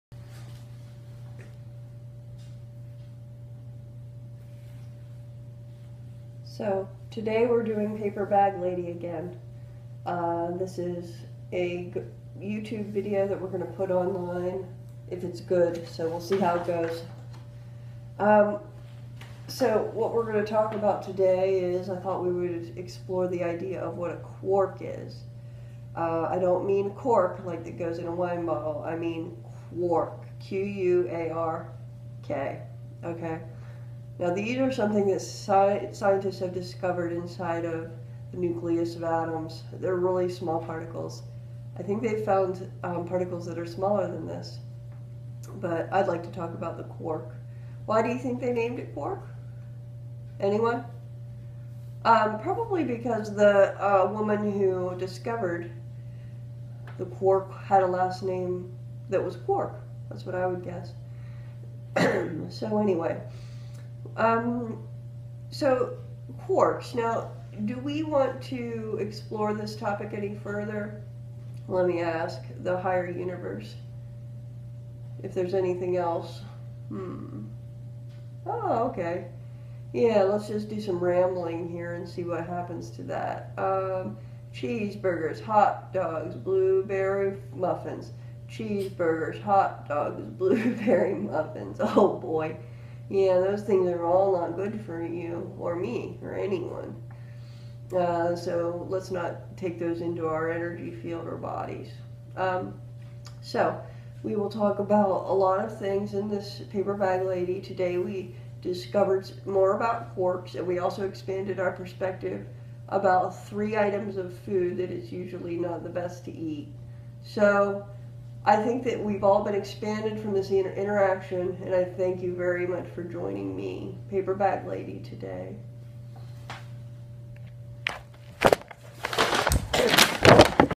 Paperbag Lady dresses up discusses quarks and food
A topic of interest to those with an inkling toward food and science.  Paperbag Lady looks great in her new get up.  Not to be missed!